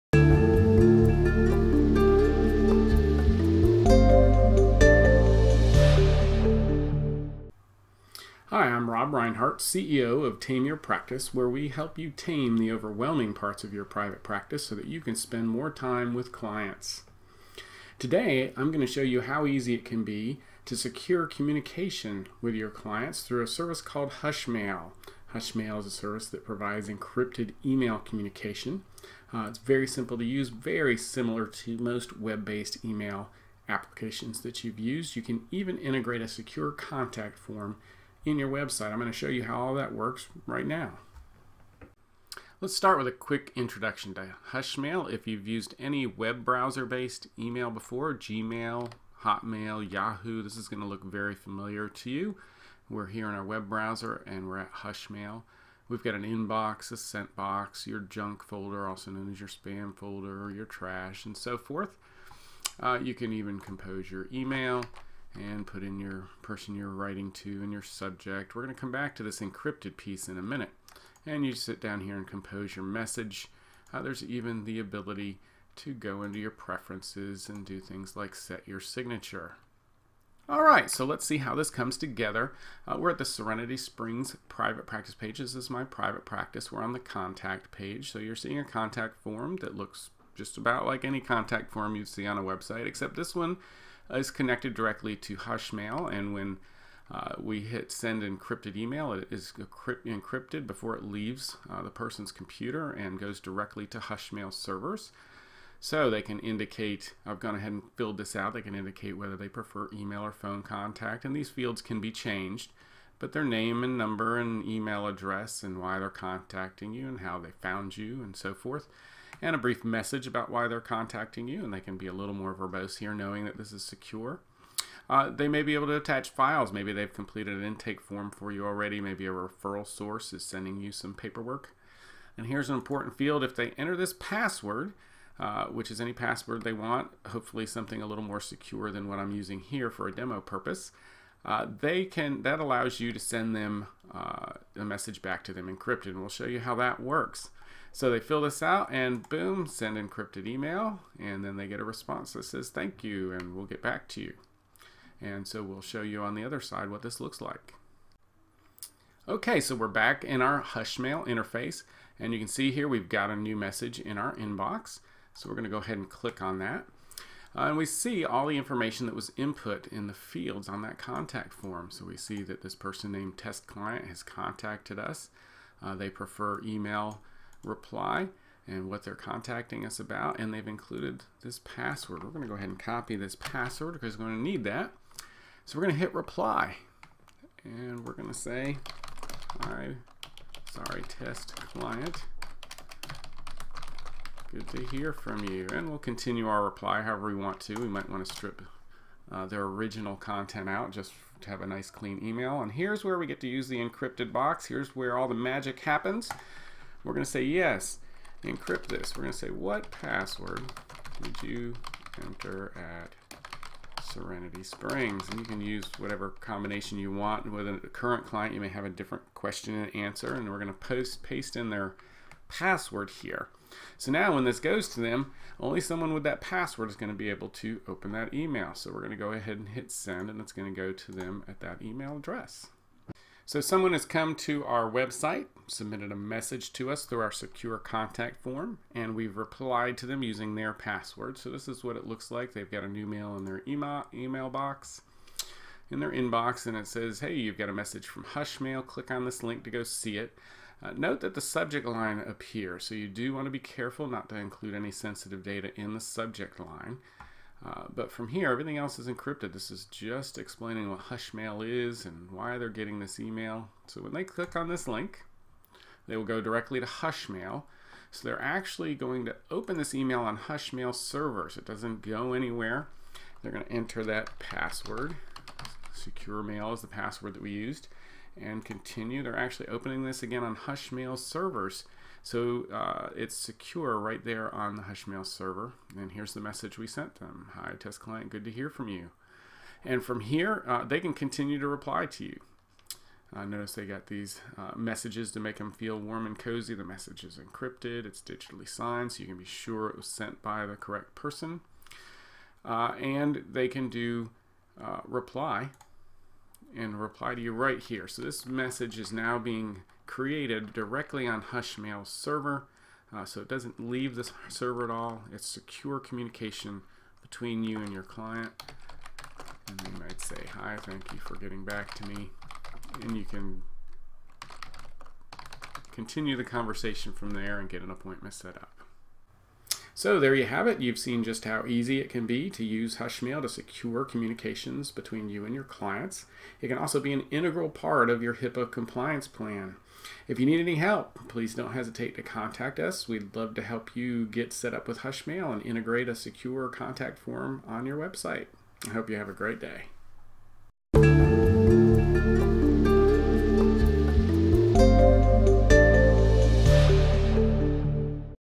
Hushmail Demo - Secure Encrypted Email - Tame Your Practice
Get a 15% lifetime discount on Hushmail through this link! If you found this information helpful and want to sign up for Hushmail, we appreciate you using this link:

See how easy it is to secure email communications with your counseling and therapy clients through Hushmail.